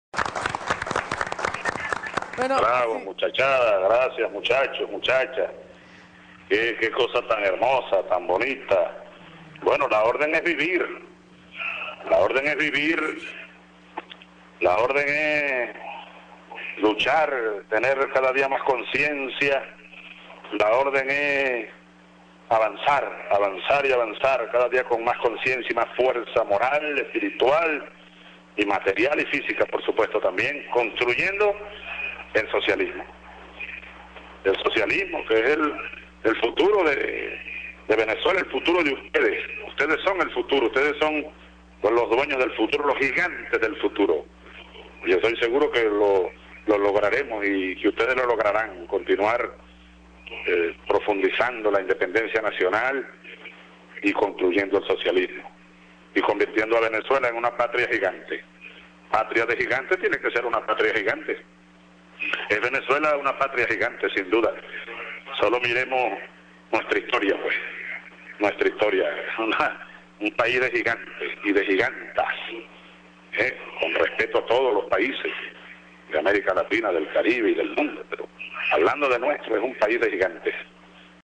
6 Abr 2012 Hugo Chávez: la orden es vivir, luchar con conciencia
Este viernes, el presidente de la República, Hugo Chávez, exhortó a la militancia revolucionaria a desplegarse y movilizarse por todo el país, para asegurar la victoria del 7 de octubre.

Manifestó que tiene fe en su plena recuperación. "La orden es vivir, la orden es luchar, tener cada día más conciencia. La orden es avanzar, avanzar y avanzar, cada día con más conciencia y más fuerza moral, espiritual, material y física", agregó.

Comentó que está seguro en que ese objetivo se logrará, para continuar profundizando la independencia nacional, la construcción del socialismo y la consolidación de Venezuela en una patria gigante.